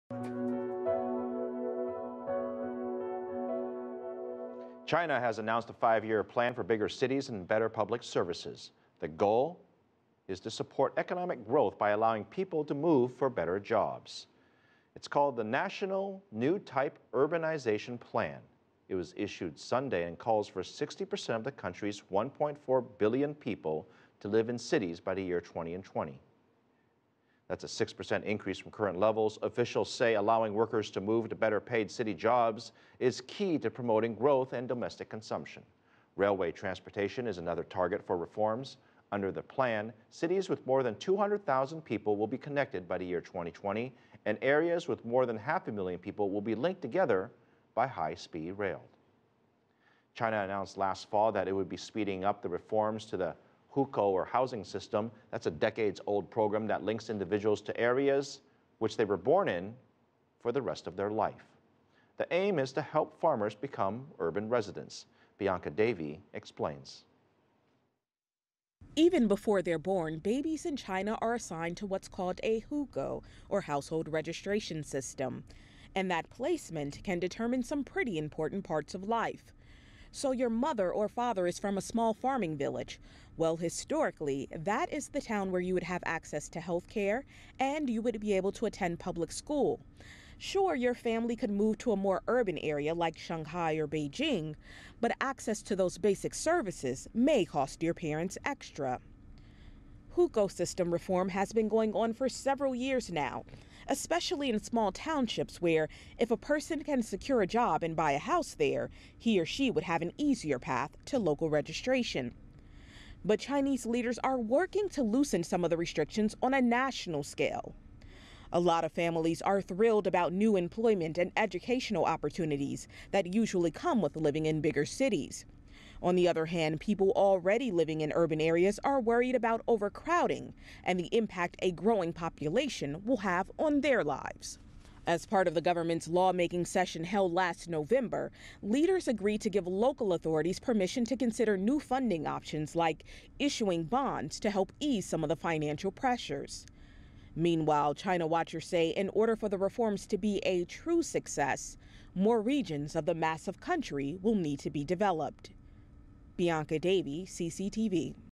Hukou System Reforms: Aiding China's Urban Development
China announced last fall that it would be speeding up reforms to the "hukou" system: a decades-old program that permanently links individuals to their place of birth. As CCTV's Bianca Davie reports, the aim is to help farmers become urban residents.